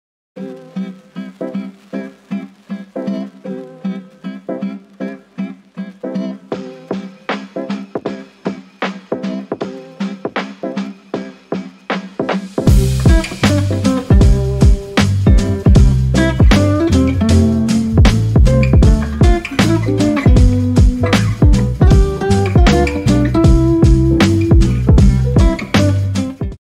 Draw and Watercolor Frosty the Snowman Holiday Icon
Learn how to draw and watercolor a holiday snowman in this process video. The doodle is perfect to use in your bullet journal, planner, embellish your holiday cards, gift tags, or any number of holiday projects.